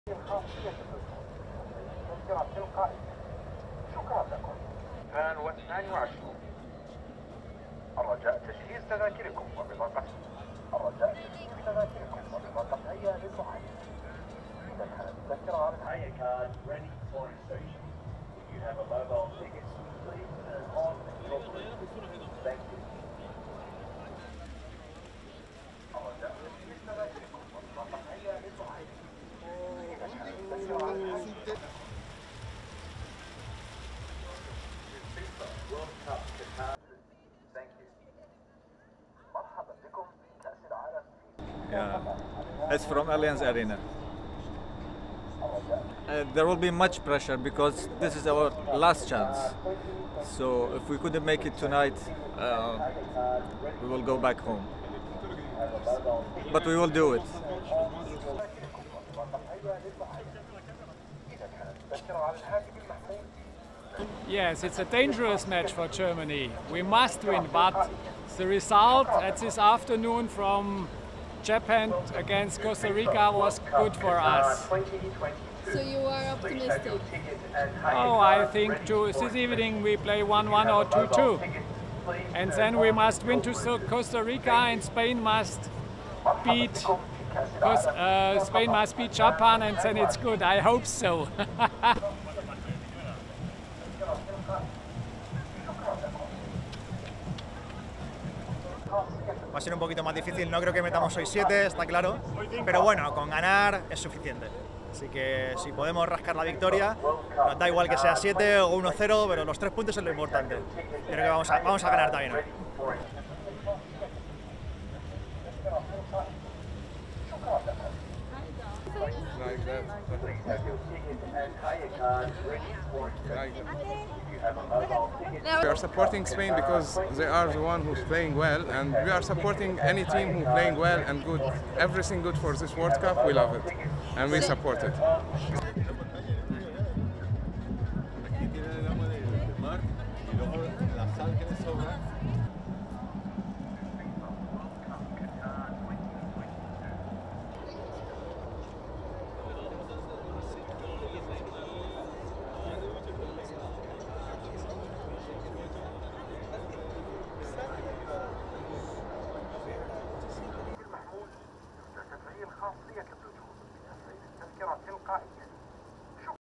Fans arrive ahead of Spain Germany game in Qatar
RESTRICTION SUMMARY:
ASSOCIATED PRESS
Al Khor, Qatar - 27 November 2022
++NIGHT SHOTS++
1. Various of fans arriving at Al Bayt Stadium
2. Mid of fans
3. Close of German team symbol on shirt
4. SOUNDBITE (English) Sitrwan Jamal, 40-year-old fan from Iraq supporting Germany:
"There will be much pressure because this is our last chance, so if we can't make it tonight we will go back home. But we will do it!"
5. Fans waving and walking up stairs
6. SOUNDBITE (English) Hajo Leuter, 58-year-old Germany supporter:
"Yes it's a dangerous match for Germany. We must win, but the result this afternoon from Japan against Costa Rica was good for us."
(Reporter: 'So you are optimistic?')
"I think this evening we play 1-1 or 2-2 and then we must win (against) Costa Rica and Spain must beat Japan, and then it's good, I hope so."
7. Spanish fans arriving
8. SOUNDBITE (Spanish) Guillermo Galindo, Spain supporter:
"It will be a bit harder. I don't think we will score seven of course, but to win is enough. It doesn't matter if it's 1 or 7, the three points is what is important. But I believe we will win again."
9. Various of Spanish fans from Lebanon arriving at stadium
10. SOUNDBITE (English) Saade Alkhoury, Lebanese fan supporting Spain:
"We are supporting Spain because they are the ones playing well and we are supporting any team that is playing well in this World Cup, we love it and support it."
11. Various of fans arriving at stadium
12. Mid of queue for entry
13. Wide of night sky
STORYLINE:
Fans arrived at Al Bayt Stadium on Sunday ahead of the decisive World Cup match between Spain and Germany.
Following Germany's loss against Japan, German fans were eager for a win.
"It's a dangerous match for Germany. We must win, but the result this afternoon from Japan against Costa Rica was good for us," said 58-year-old Germany supporter Hajo Leuter.
Sitrwan Jamal, a 40-year-old Germany fan from Iraq said: "There will be much pressure because this is our last chance, so if we can't make it tonight we will go back home. But we will do it!"
On the other side, Spain is coming from a strong 7-0 victory against Costa Rica.
Another loss for the Germans against Spain would virtually end their chances of reaching the knockout stages.